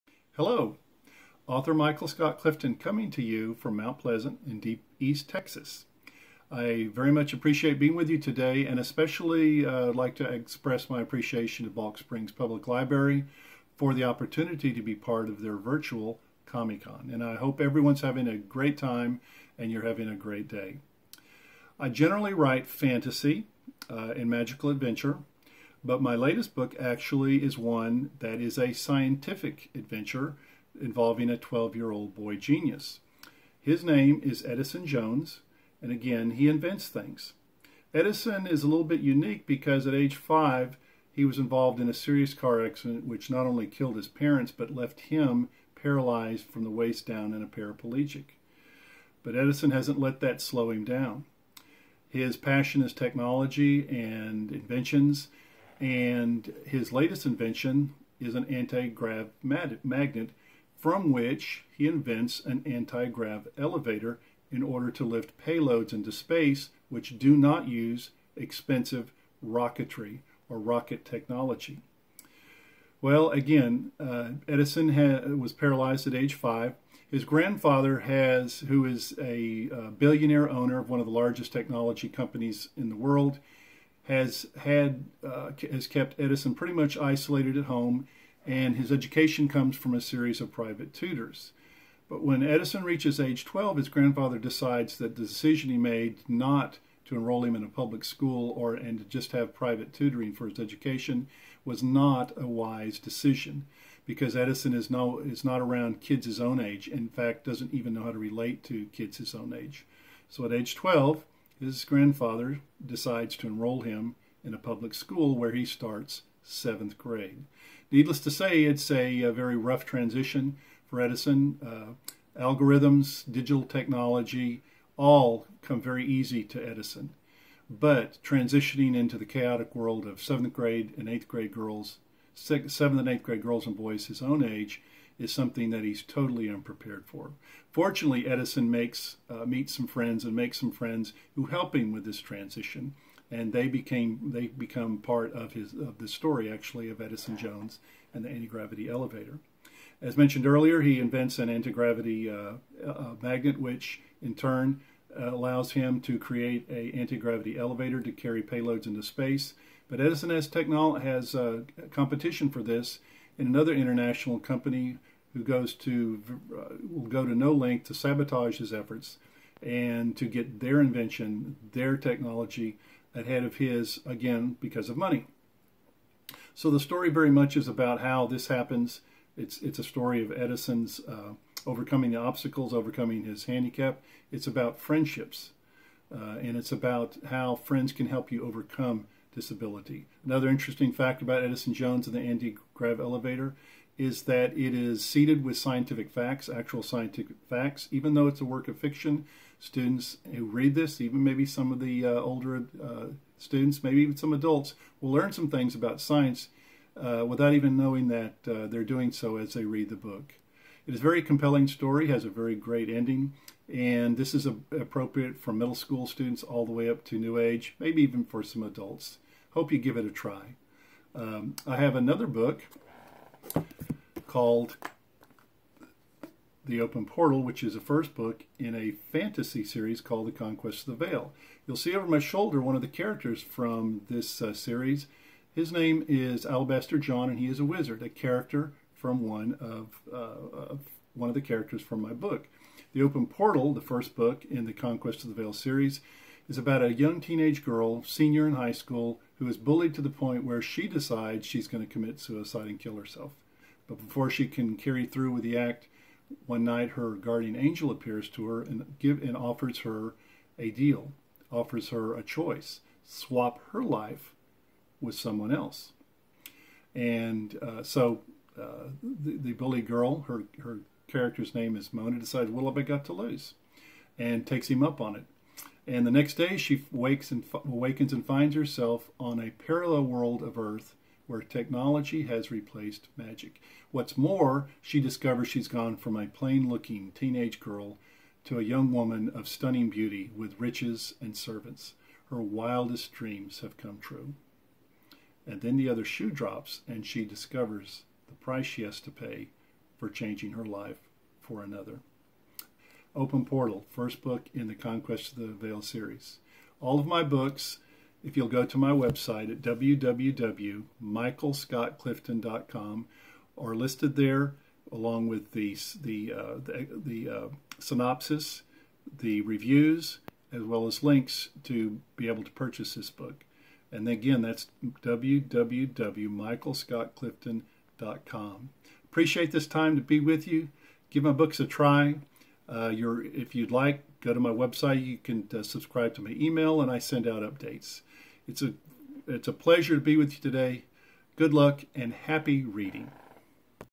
Balch Springs Comic Co
My synopsis of "Edison Jones and the Anti-Grav Elevator" and The Open Portal" for the Balch Springs Library Comic-Con.